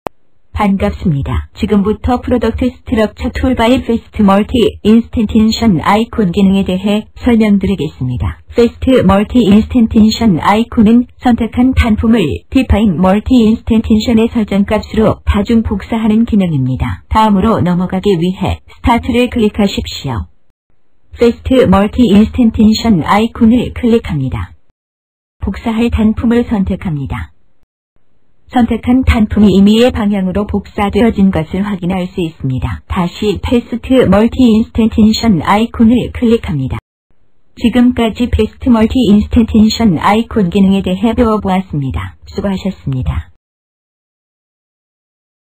CATIA V5 - ASSEMBLY - Fast multi instantiation [카티아스쿨]
카티아스쿨입니다. 이번 시간에는 CATIA V5 ASSEMBLY에 Fast multi instantiation 기능에 대해 배워보도록 하겠습니다.